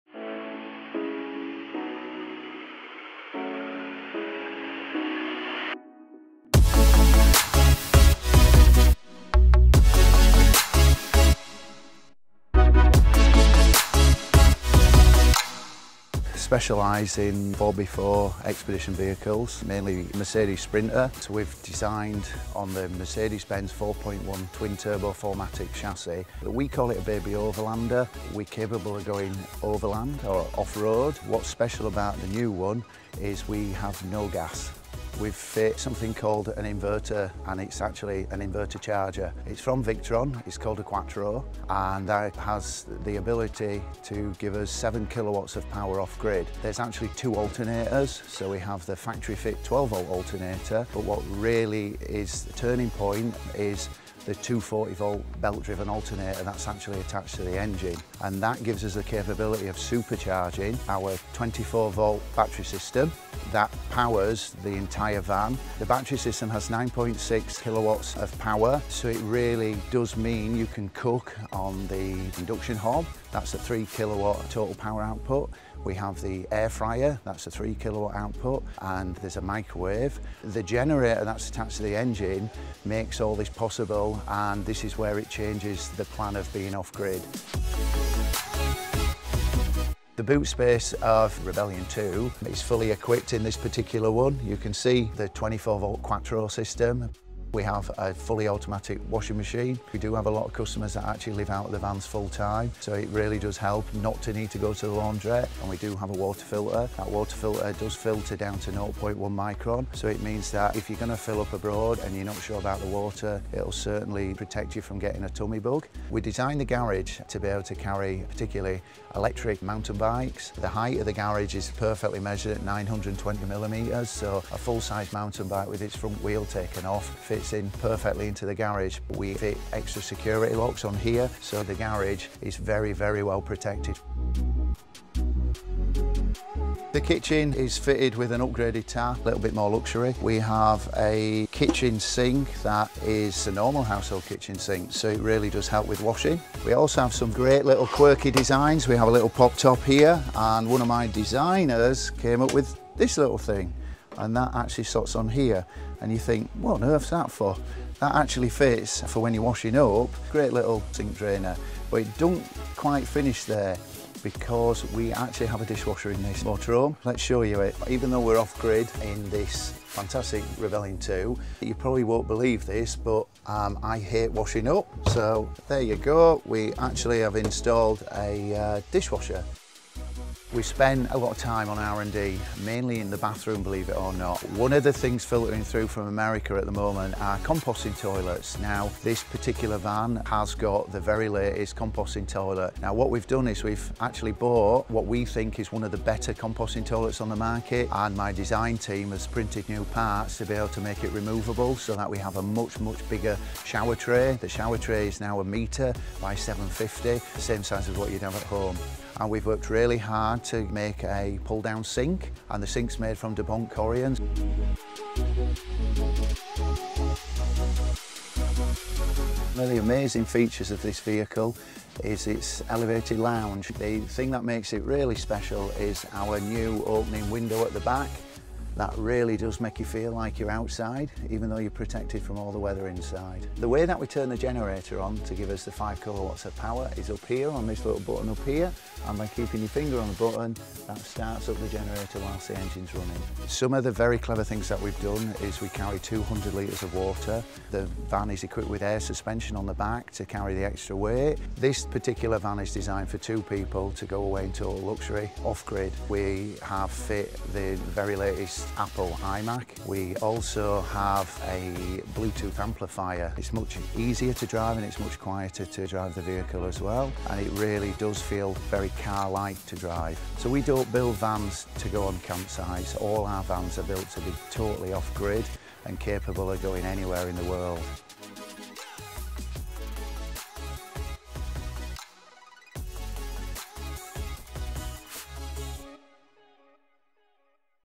4X4 Sprinter 4matic Campervan
The Rebellion by RP Motorhomes - This 4x4 Overland Motorhome is the ultimate adventure vehicle which can take you anywhere you want to go.  This 4x4 Mercedes Benz 4matic Sprinter Van Conversion can be off grid, with this gas free option and the incredible 24 volt Battery system.   This Rebellion 2 4x4 Campervan is fitted with all of your home comforts - an air fryer, microwave, dishwasher, iMac and even a washing machine! With a huge rear garage, you will be able to take all of your sporting equipment such as mountain bikes, SUPs, surfboards and with the external shower point, you can shower off outside!